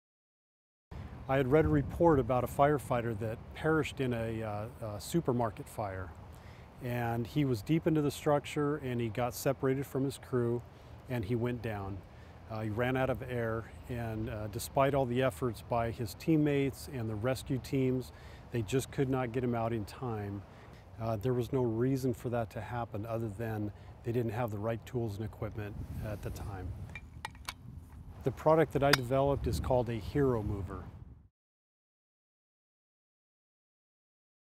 Joe's Story part 1: Expanding dreams and ideas
Joe Noonchester, a firefighter invented a life saving device for his fellow firefighters and talks about the importance of help in getting his product ready. The NWIRC has been able to “take my idea and dreams and expand them so much further… they really brought my idea to life.”